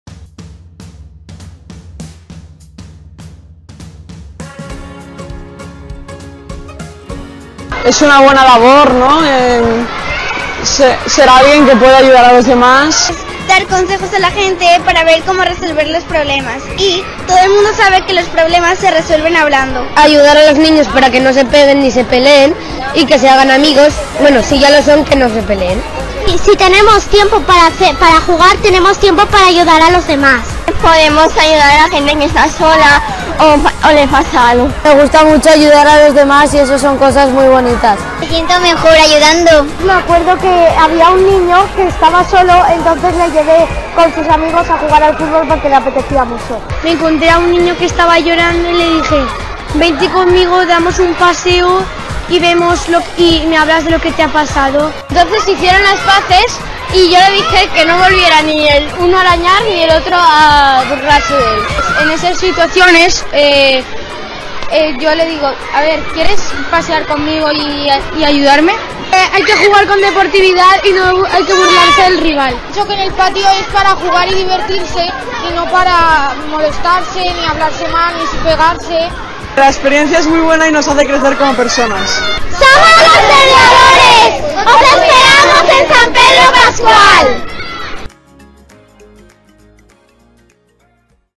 mediación | Entrevista a los
Una vez pasado el primer trimestre el alumnado de 5º y 6º que han sido mediadoreSPP nos explican su experiencia y el trabajo realizado con sus compañeros y compañeras.

Una gran labor que se nota en los recreos. ¡Gracias por vuestra dedicación y compromiso!